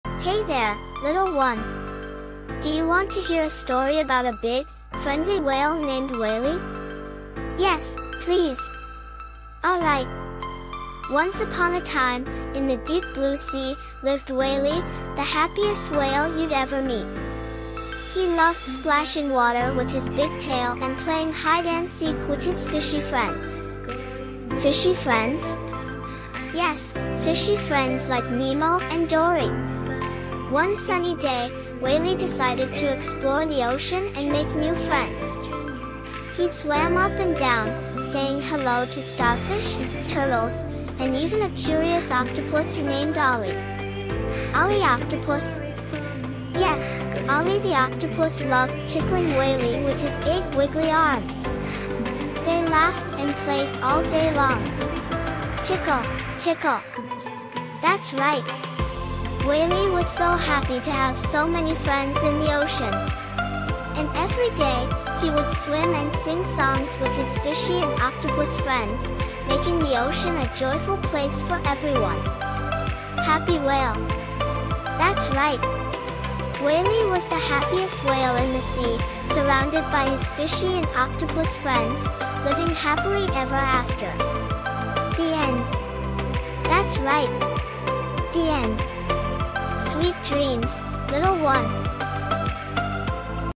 Story for kids
it's a story of a whale fish
short story for kids bedtime story